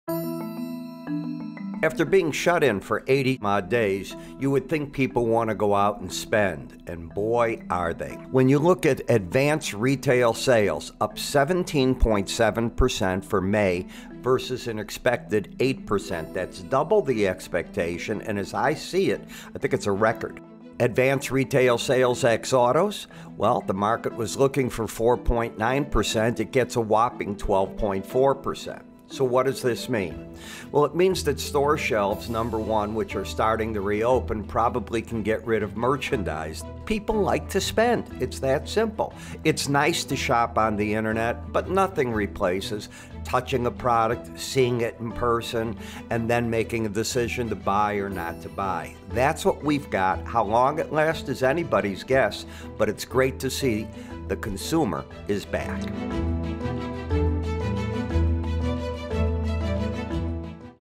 U.S. retail sales soared to 17.7% in May as lockdown ease
American consumers are getting their groove back as retail sales jump in May suggesting a quick recovery. Insights via @cmegroup